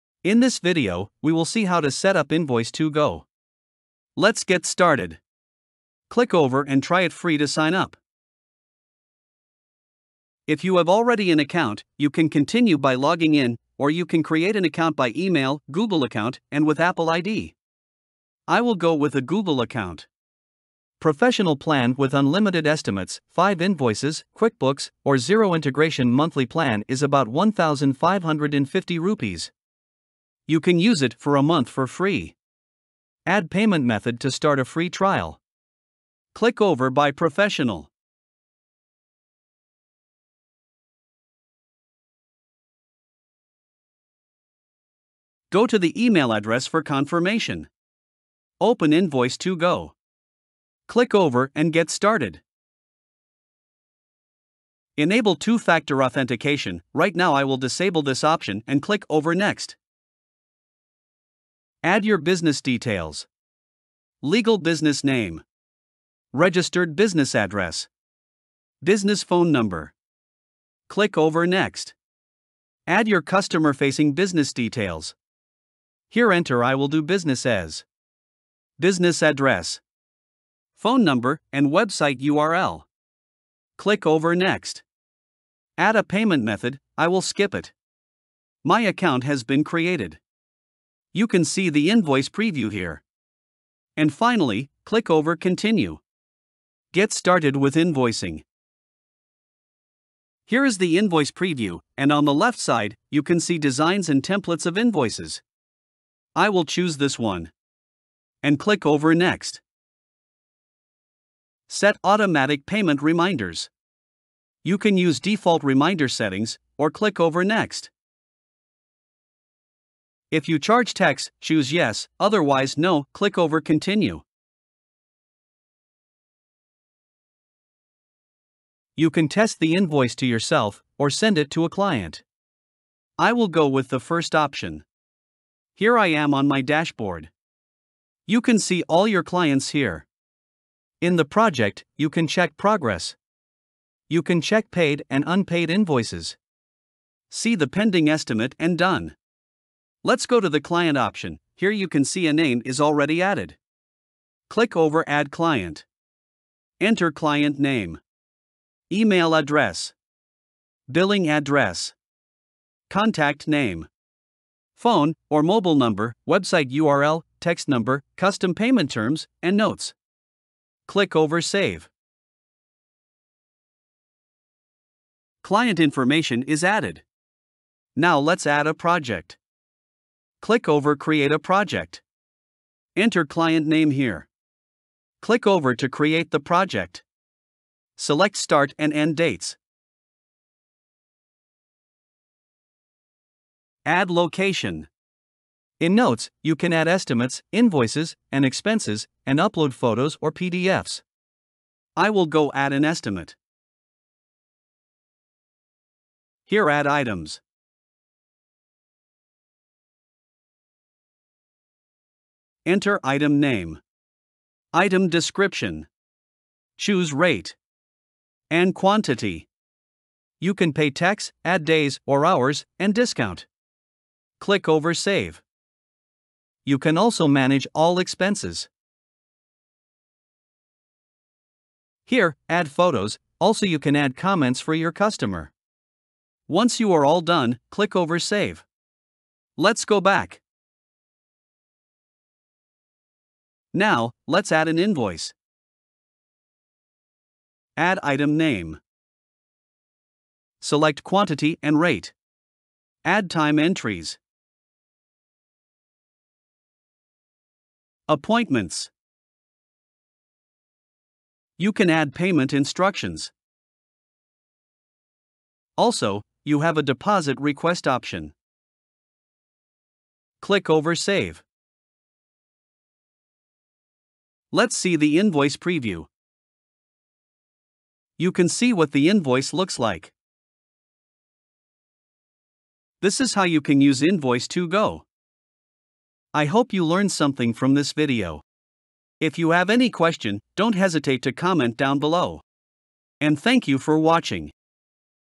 How To Use Invoice2go 2024 | Invoice2go Tutorial For Beginners 2024 {Step-By-Step Guide}
In this comprehensive guide, we'll walk you through the setup process with our Invoice2go Tutorial Setup 2024. Whether you're a seasoned user or a beginner looking to navigate the latest enhancements, this tutorial is designed for you. Discover the essentials of how to use Invoice2go 2024 to streamline your invoicing and billing processes. Join us on this journey, and soon you'll be maximizing the potential of Invoice2go for your business needs.